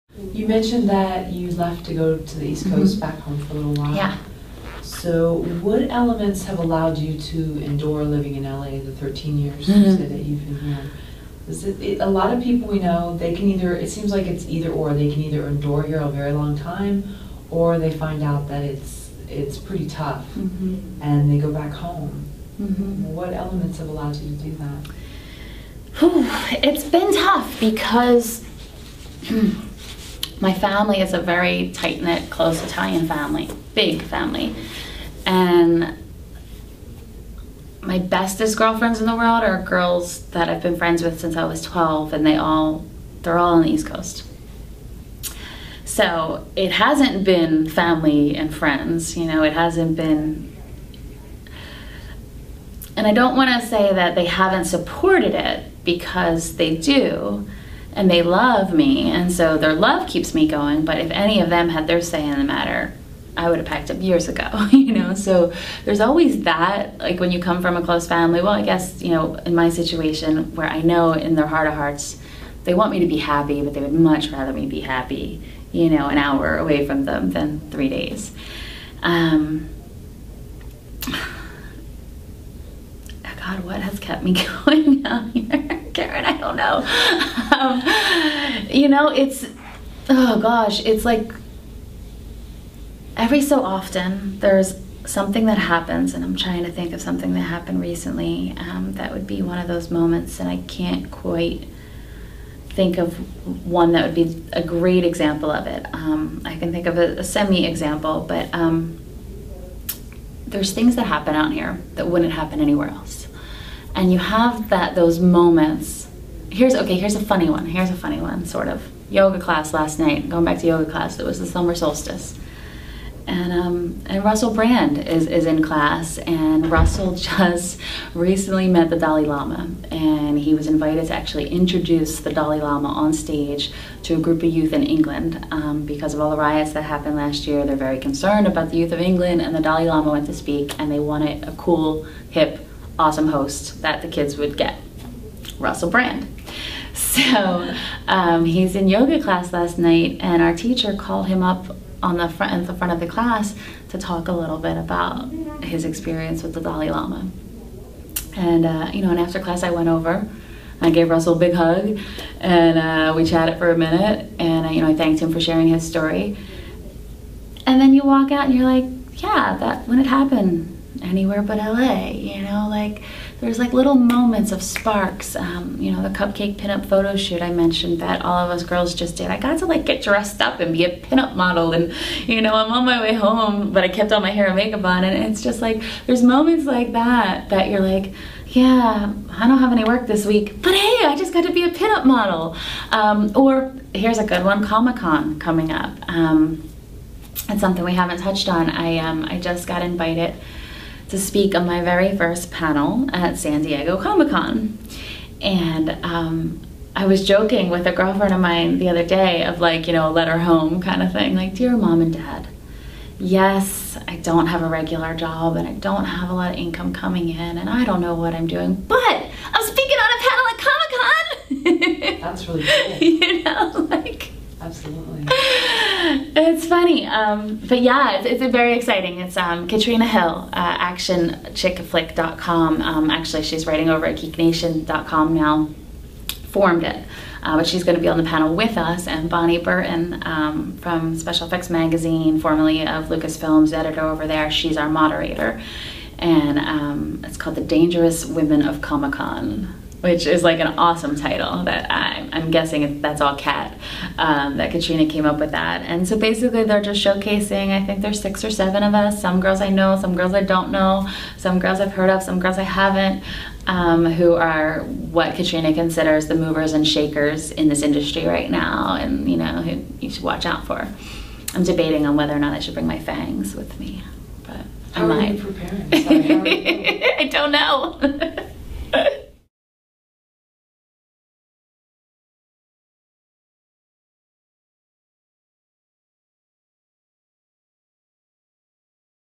Hollywood, Russell Brand and Comic-Con by Leah Ann Cevoli
Actress Leah Ann Cevoli (Robot Chicken, Deadwood) talks about what has kept her going in Los Angeles.  It often comes down to moments that wouldn't happen anywhere else.  She tells us a Russell Brand / Dalai Lama story, being a pin-up model (Cupcake Quarterly), and being invited to speak at Comic-Con.